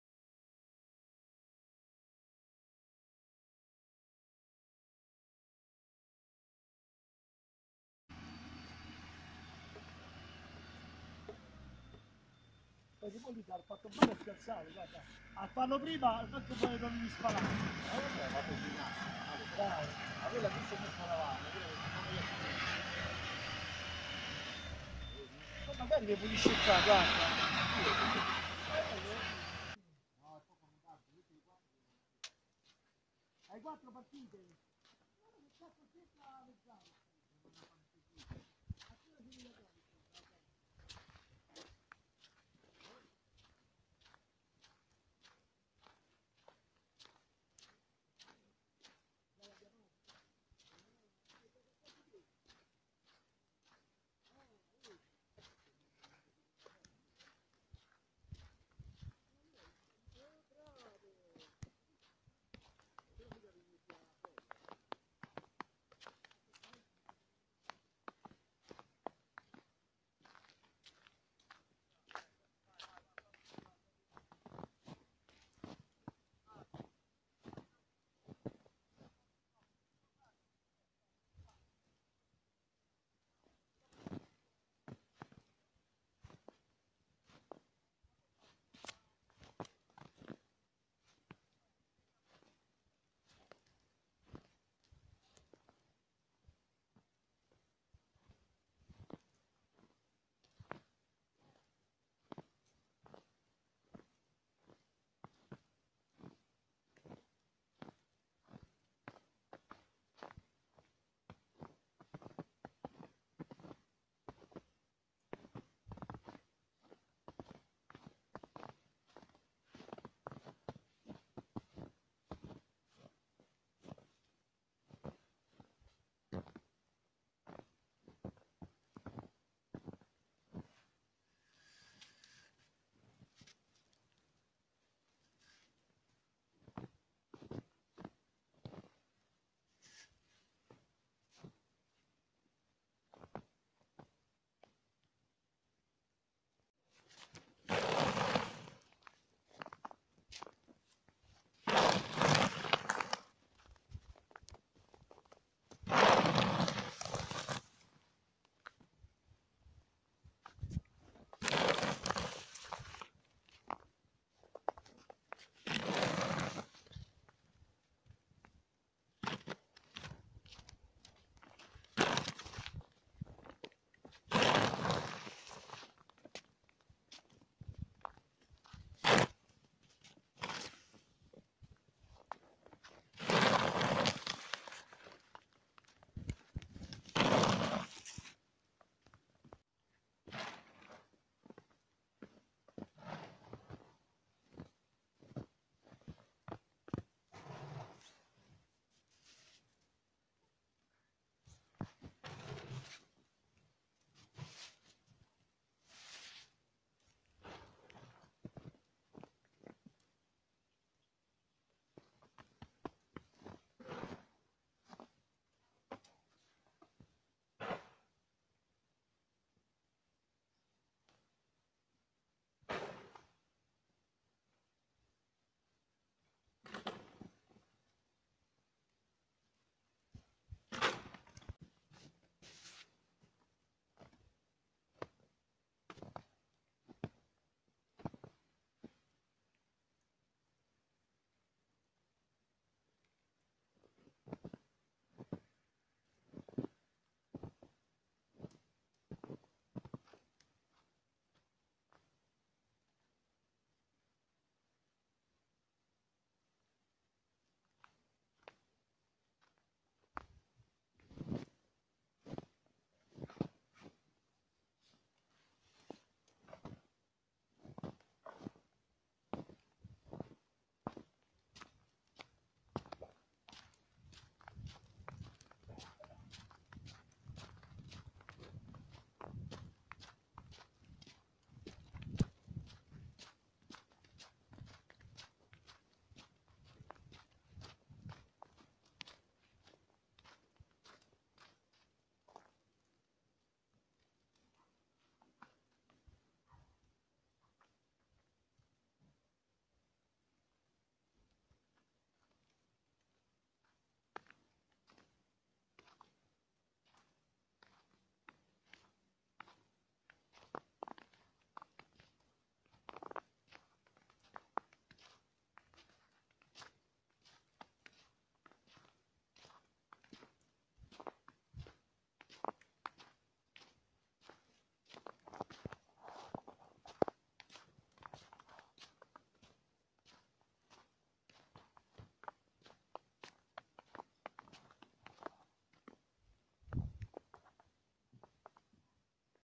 NEVE AQUILA; CENTRO ITALIA COLPITO DALLA NEVE, IL PEGGIO È IN ARRIVO!!
20 cm di neve abbondanti !!